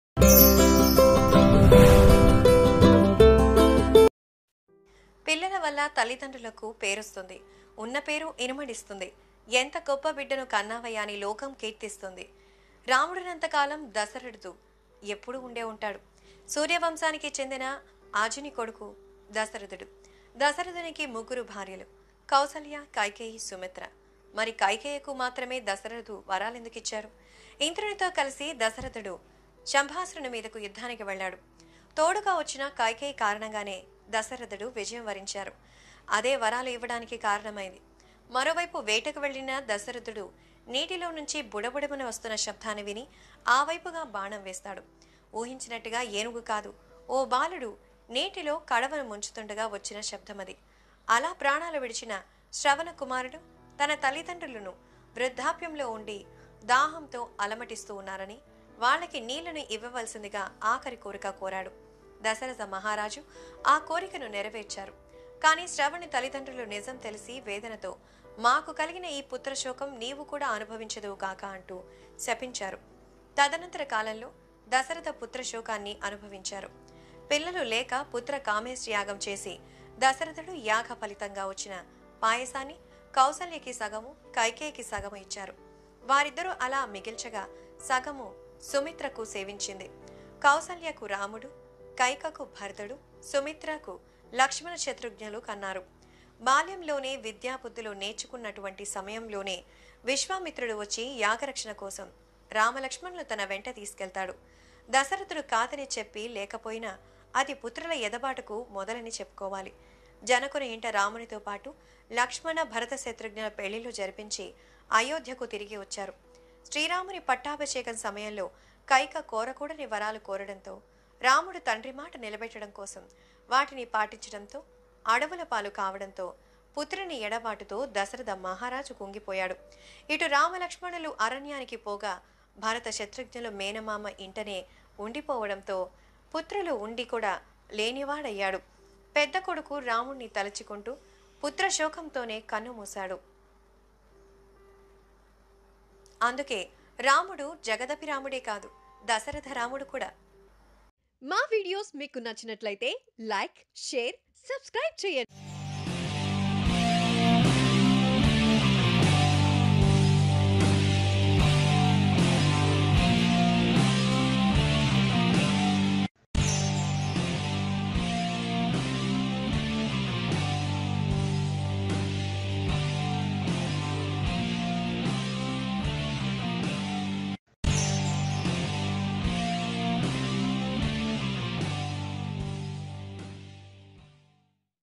The Story Of King Dasharatha & Kaikeyi In Ramayanam | Hindu Mythological Stories | News Mantra
The Story Of King Dasharatha & Kaikeyi In Ramayanam. King Dasharatha & Kaikeyi bonding are explained in this video.